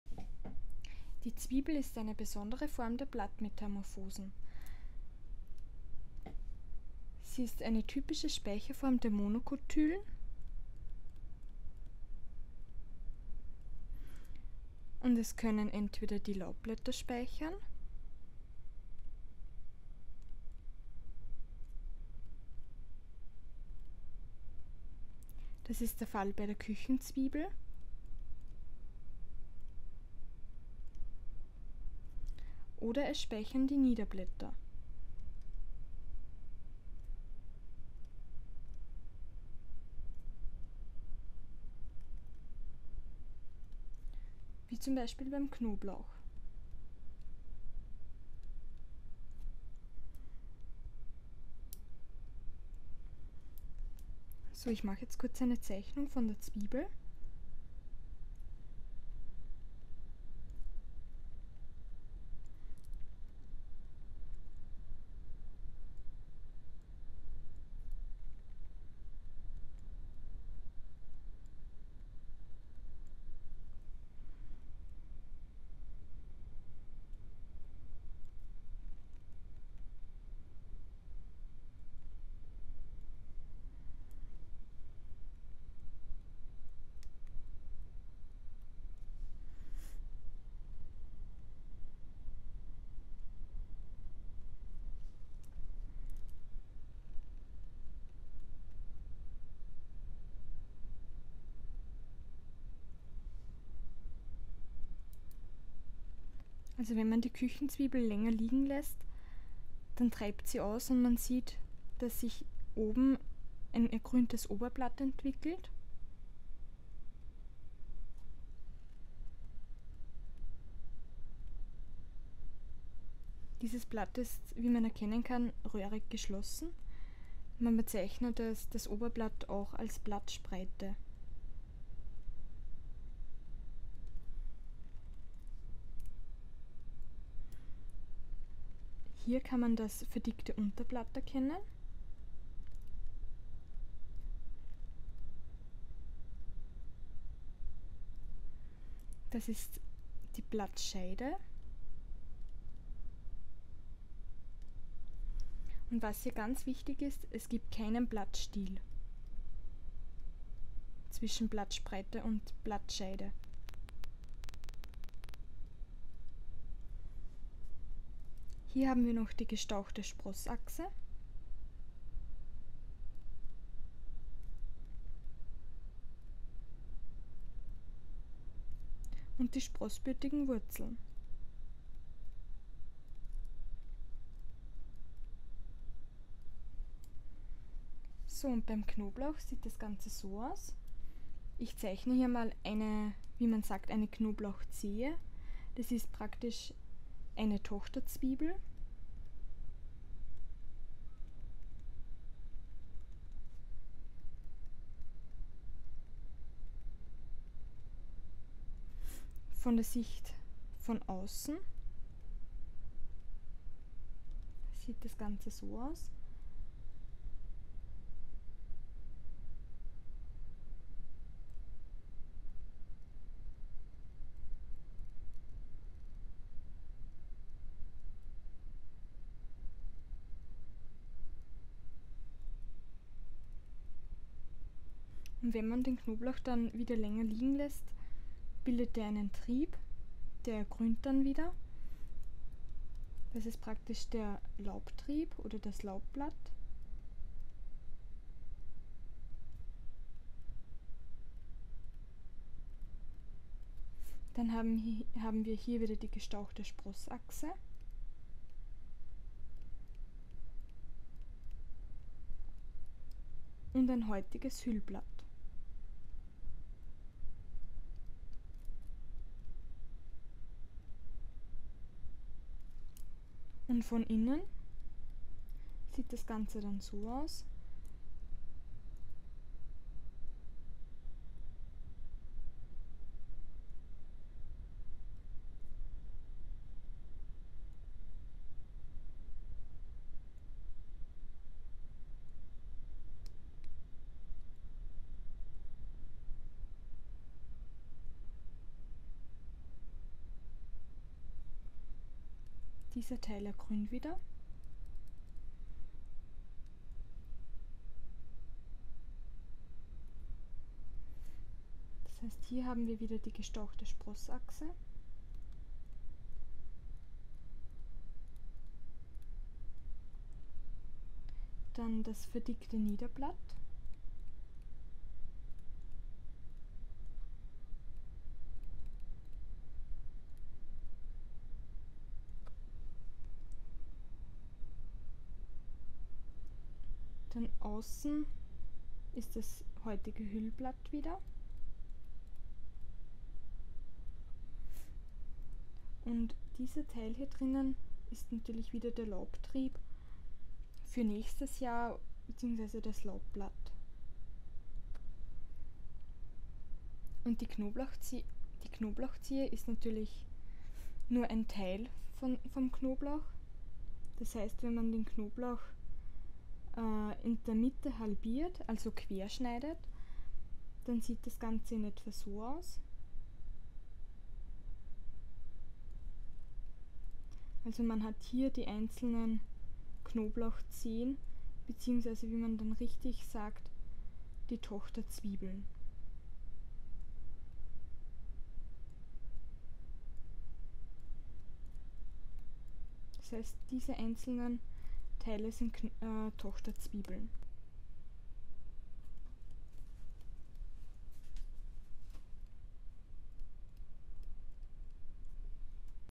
Blattmetamorphose - Zwiebel
Speicherform der Monokotylen
Vergleich Knoblauch - Küchenzwiebel